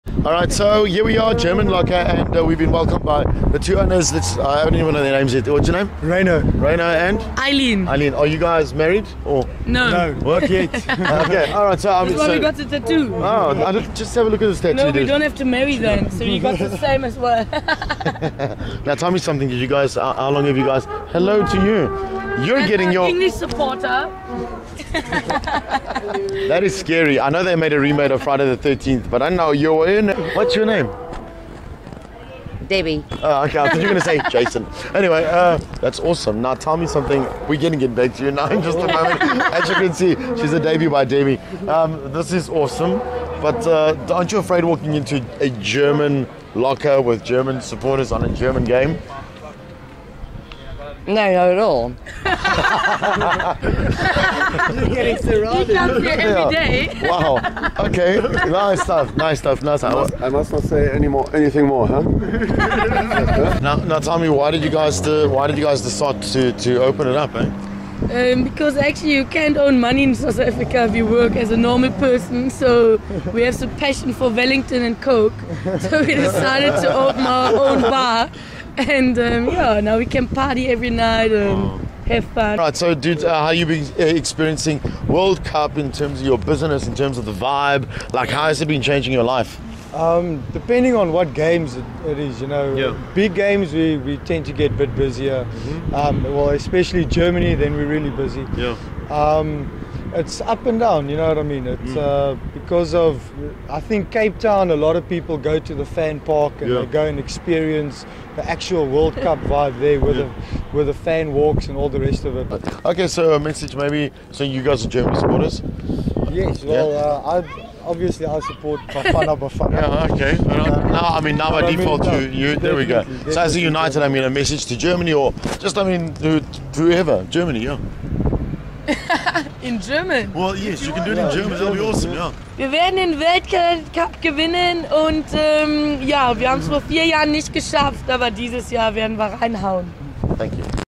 Van in a Van: The German Locker
Visiting the German Locker and chatting to the owners.